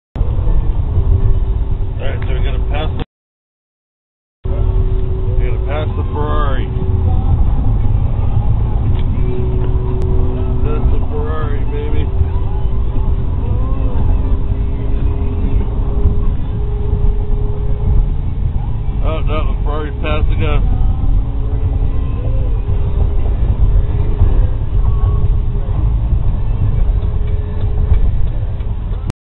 racing a ferrari
we passed him but he took off fast at the end